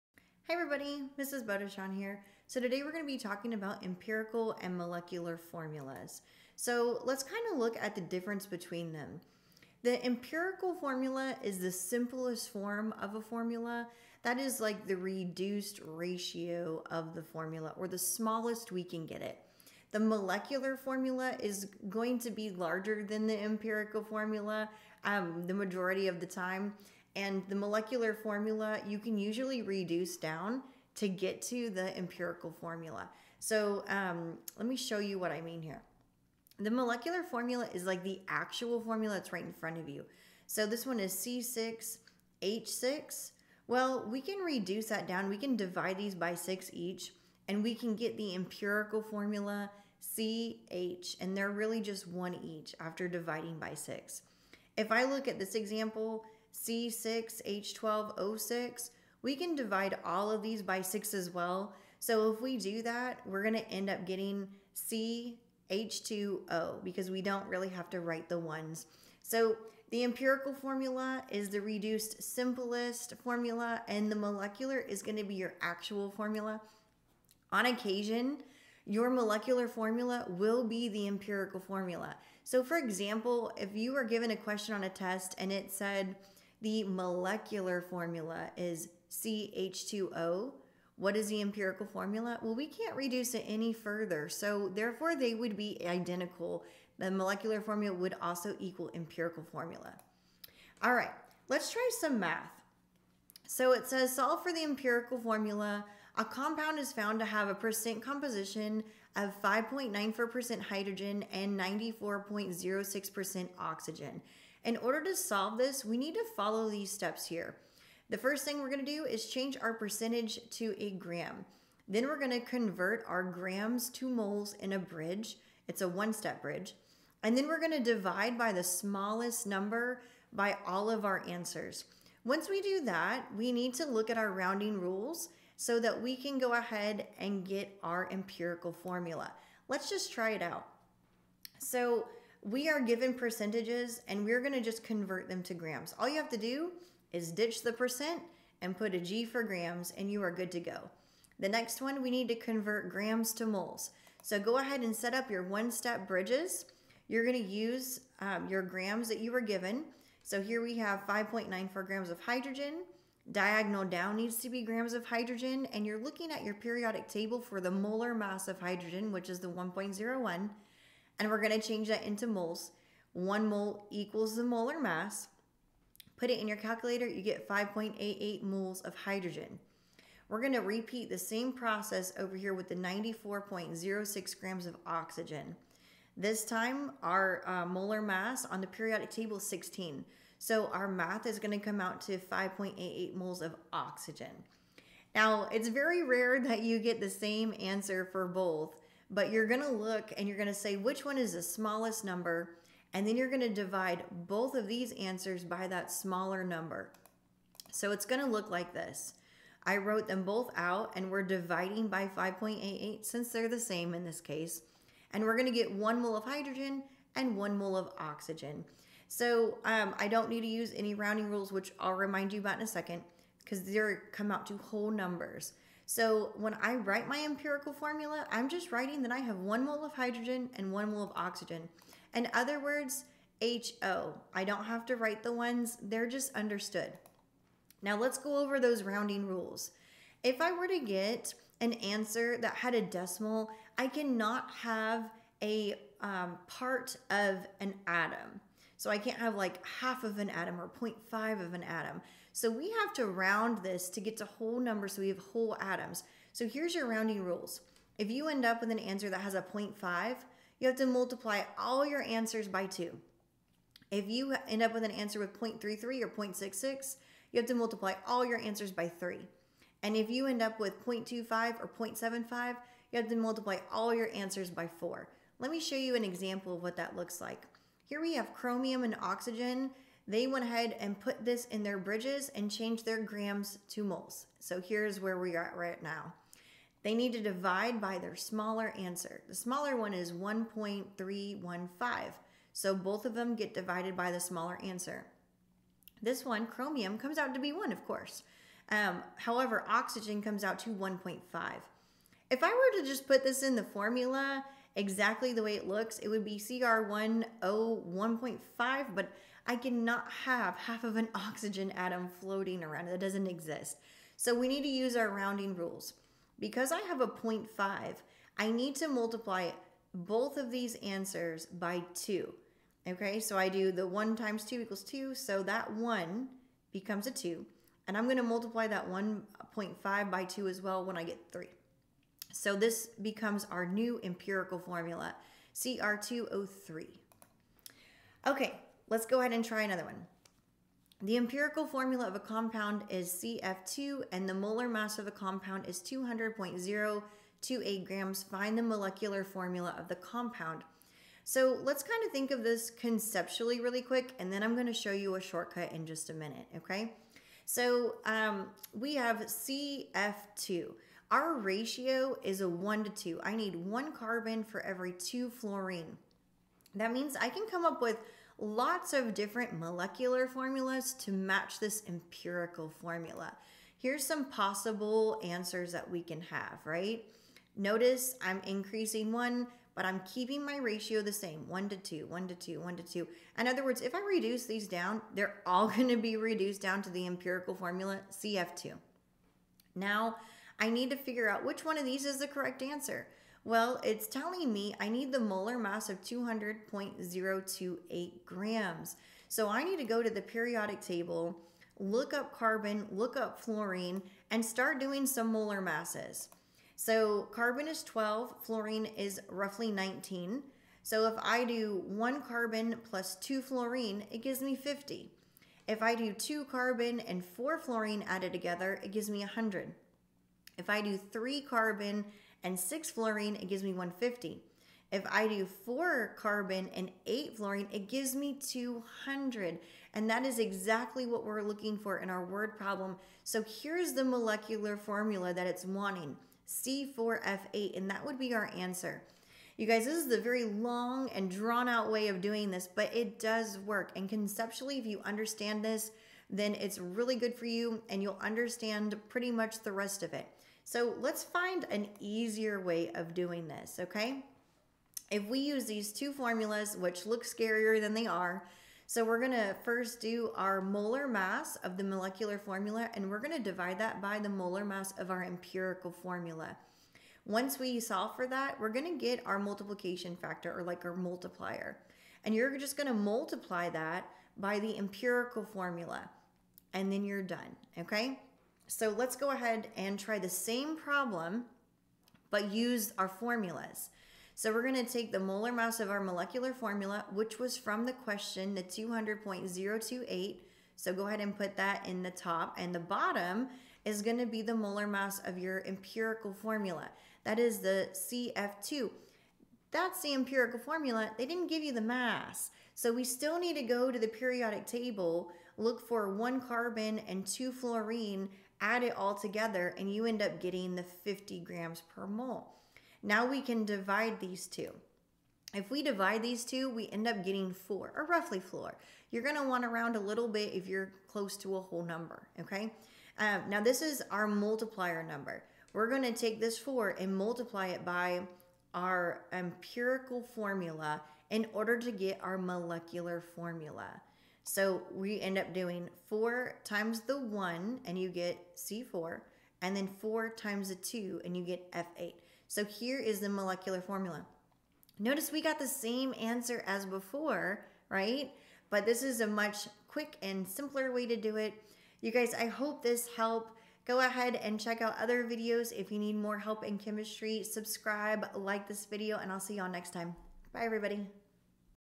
Empirical and Molecular Formulas | Concepts and Math Examples Made Easy | Step by Step Instruction
Mrs. Bodechon will teach you all about Empirical Formulas and Molecular Formulas, including what they are and examples of each. Then she will teach you how to solve mathematically for them both with easy step by step instructions.